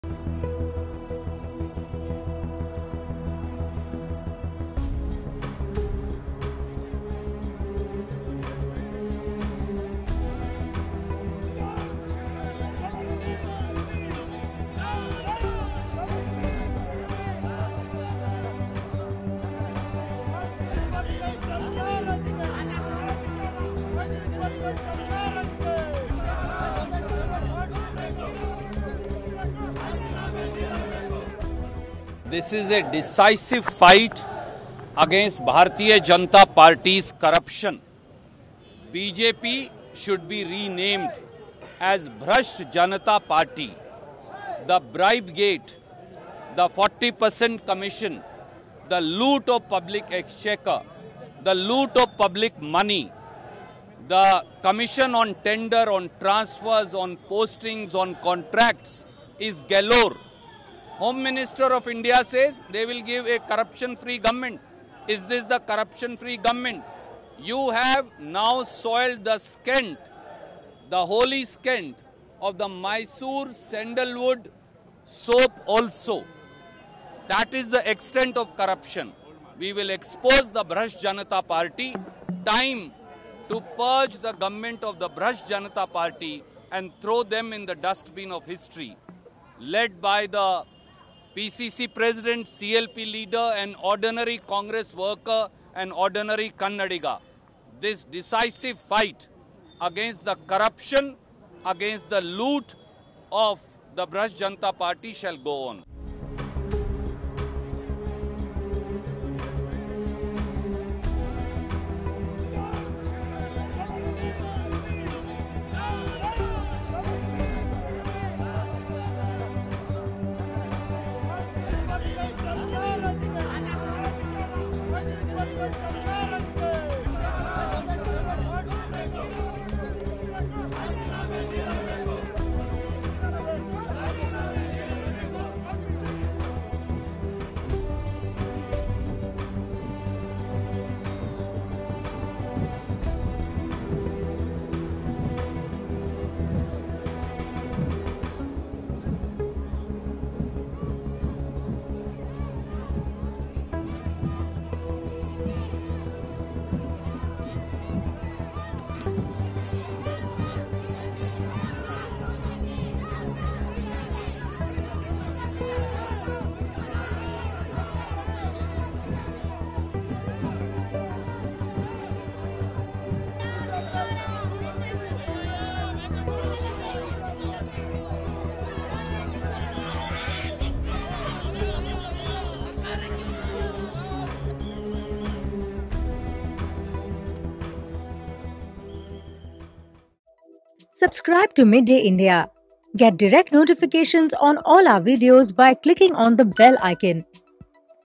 Fight Against Corrupt BJP Shall Go On, Says Randeep Surjewala
Congress leader Randeep Surjewala attacked Karnataka BJP government on March 04 and accused the government of being indulged in corruption. He said Karnataka government is a ‘corrupt government’ and BJP will be thrown out from power. Congress leader affirmed that the “decisive fight against the corrupt BJP shall go on.”Randeep Surjewala